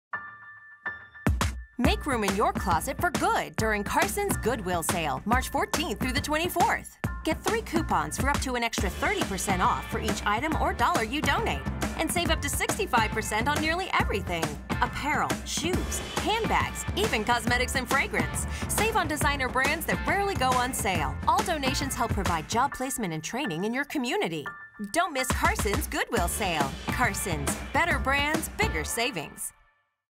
Win a $500 shopping spree AND do something good!
Make room for good in your closet and in your life! We would love for you to shop our Goodwill Sale going on now through March 24. Shop with us now and support Goodwill!